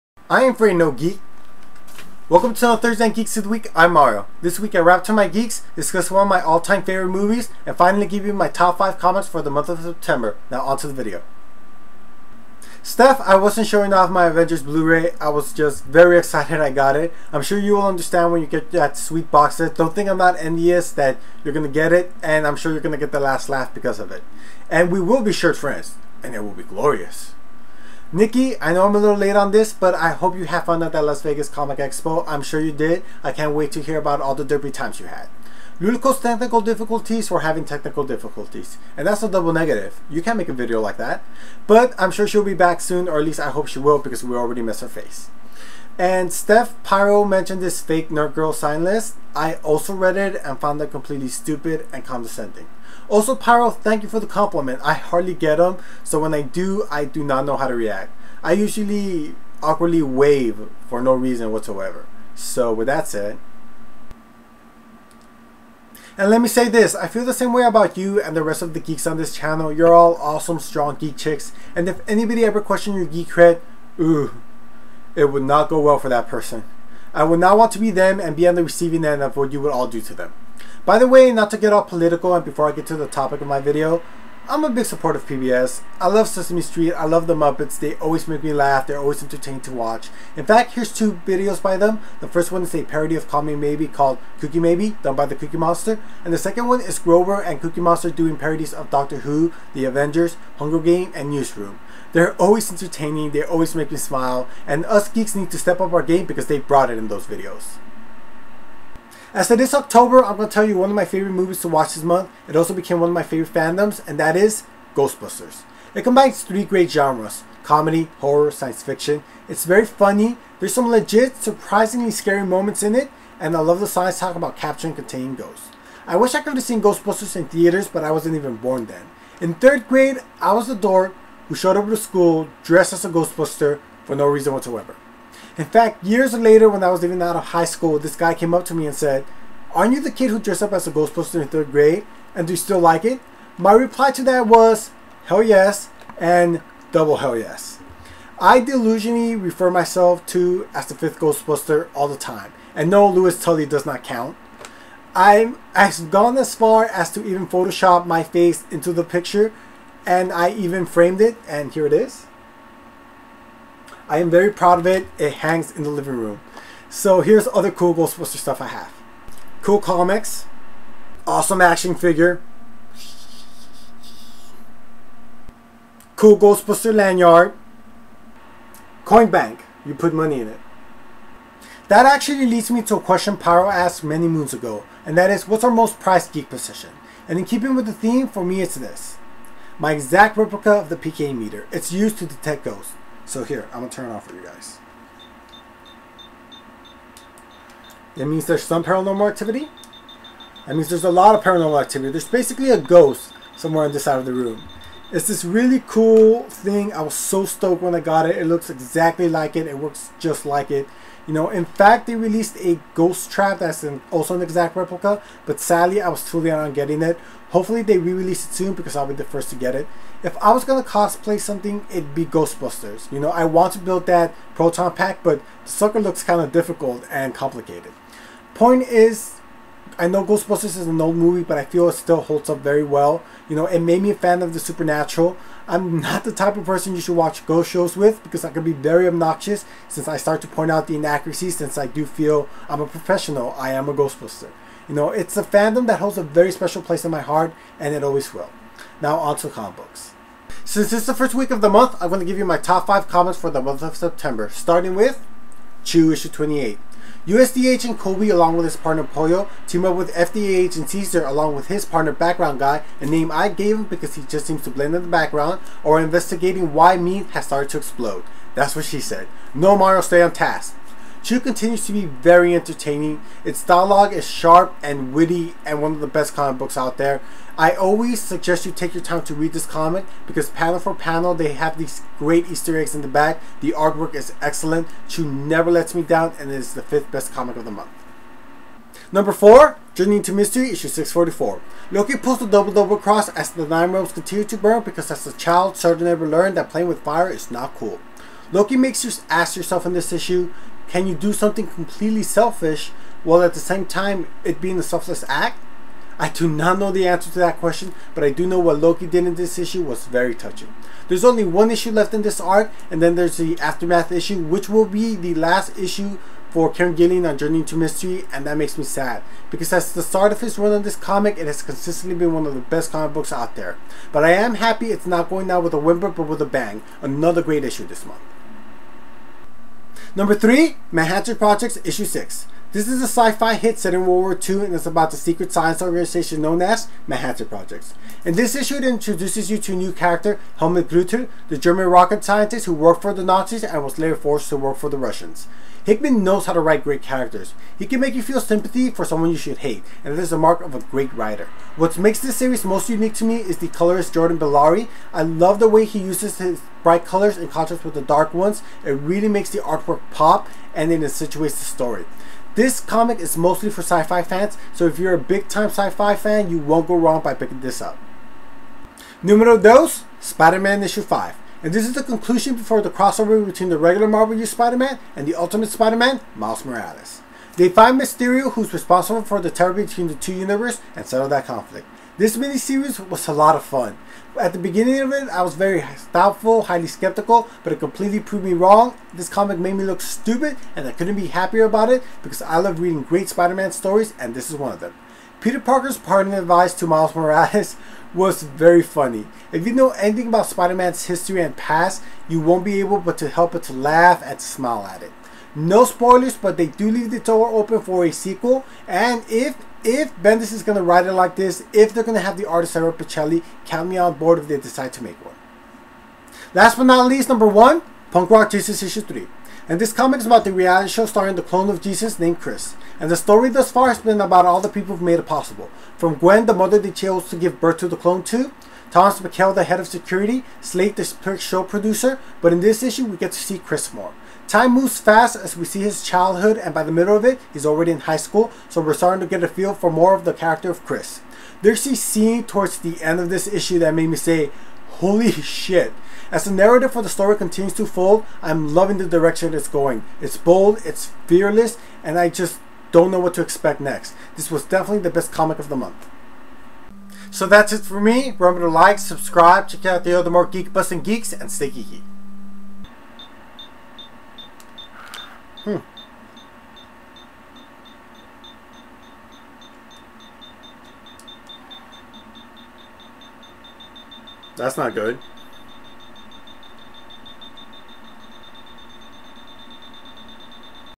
The Fifth Ghostbuster! Top 5 Comics For September.
*I completely geek out over Ghostbusters!
*If you noticed the background I have already decorated for Halloween
*Top 5 Comics for the Month of September.